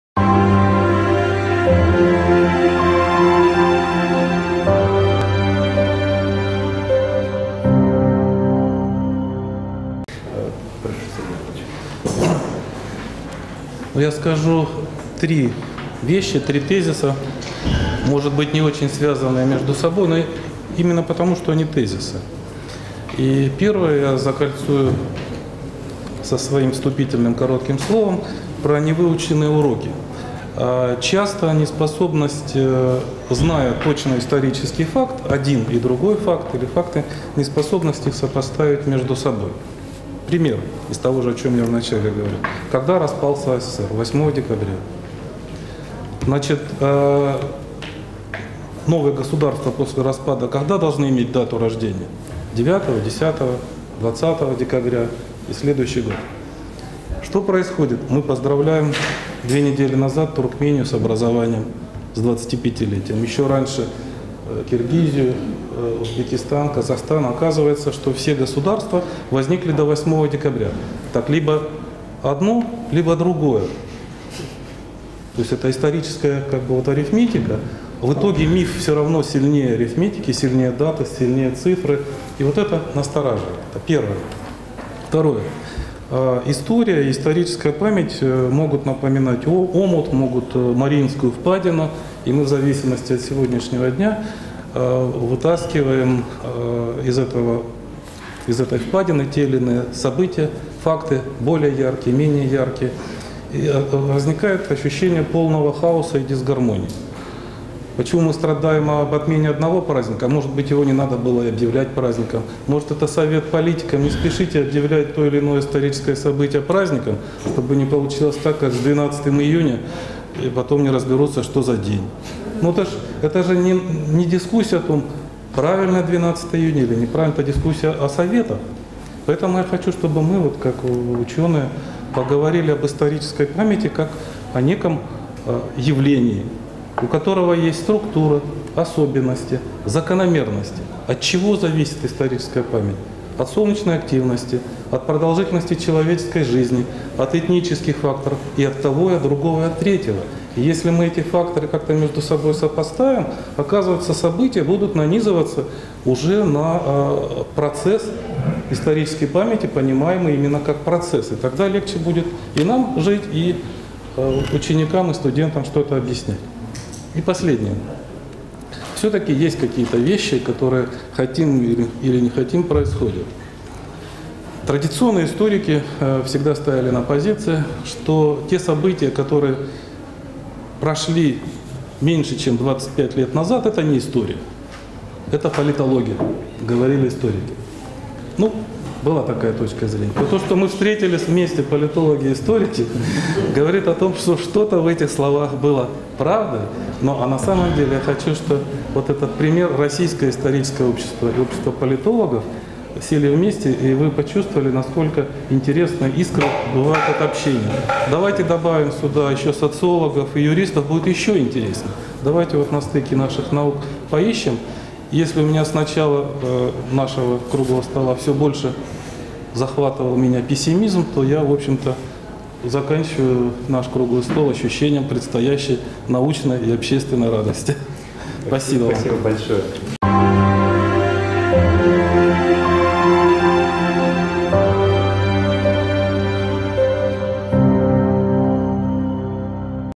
Второй Съезд Российского общества политологов. Спикер - Сергей Шахрай
Второй Съезд Российского общества политологов «Российская политика: повестка дня в меняющемся мире». Круглый стол "Политика исторической памяти в современной России: возможности и ограничения формирования и развития российской идентичности". Спикер - Сергей Шахрай, Председатель Правления Российского исторического общества, проректор МГУ имени М.В. Ломоносова.
Казанский федеральный университет 10-13 ноября 2016 года